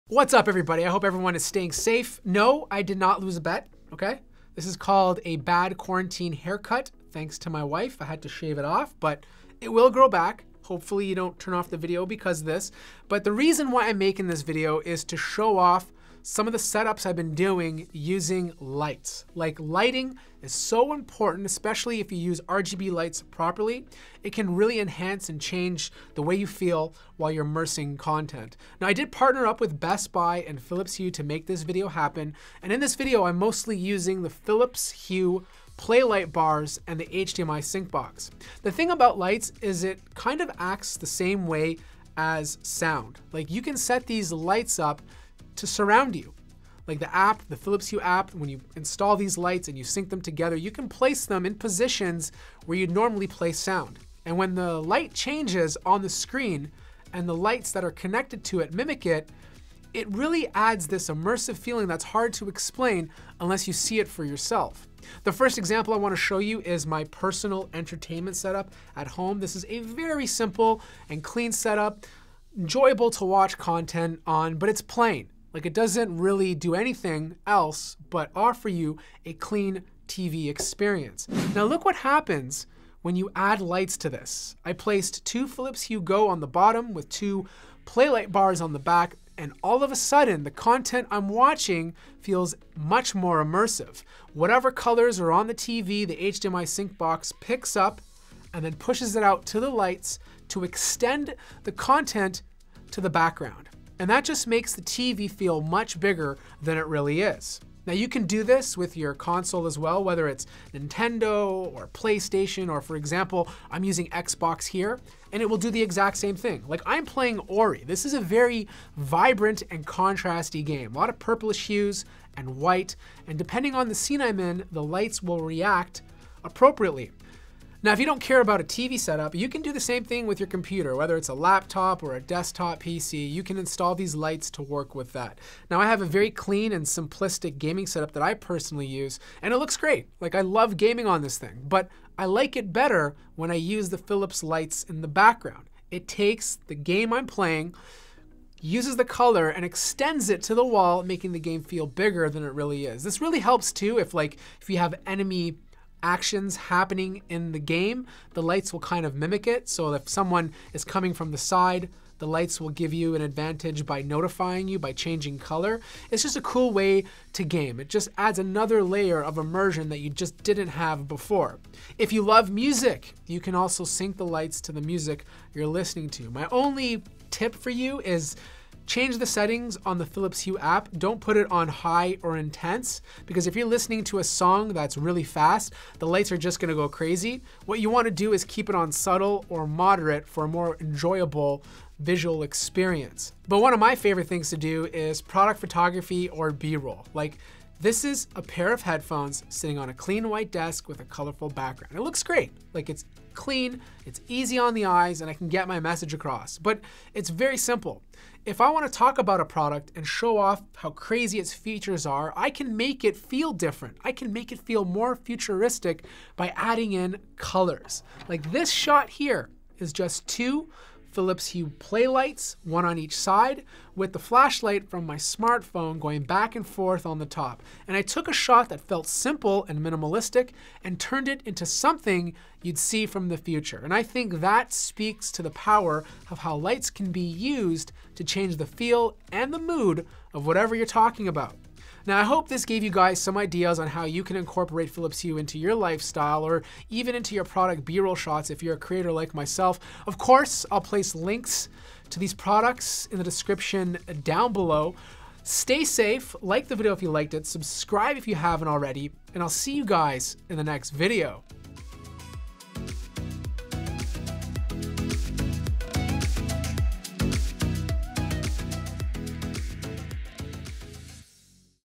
How To Make Every Room Setup Better with LED Lights!
RGB LED lights get a bad rep when they're overused but when used properly they can completely change the way photography, room setups and entertainment spaces look and feel. Here's a video on how I used a few products from Phillips Hue to make my PC Gaming setup, Entertainment space and photography shots look better!

Best Buy Canada:
Philips Hue Play Smart LED Light Bar
Philips Hue Play HDMI Sync Box
Philips Hue GO

Best Buy US:
Phillips Hue Play Smart LED Light Bar
Phillips Hue Play HDMI Sync Box
Phillips Hue GO